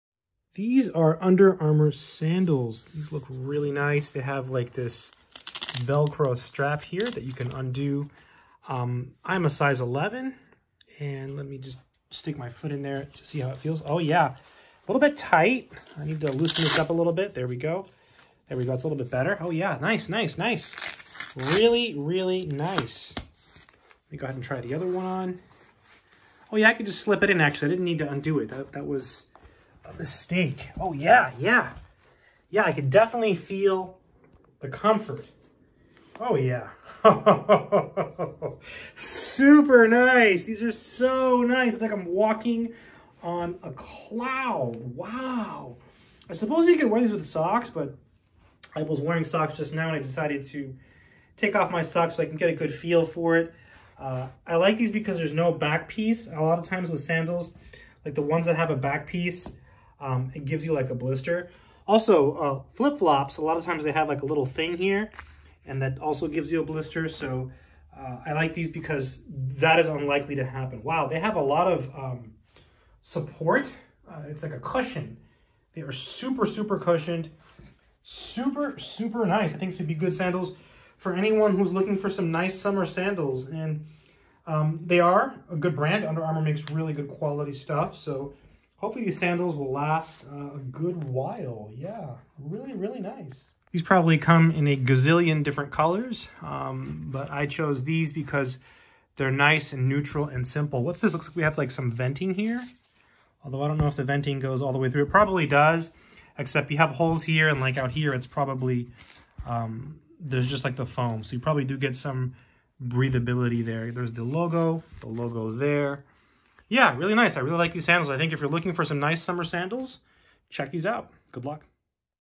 Under Armour Men's Ignite Vi Graphic Sandals
In this video I talk about some very nice sandals. These are by Under Armour and they are called the Under Armour Men's Ignite Vi Graphic Sandals. I hope this is helpful.

These are the sandals on amazon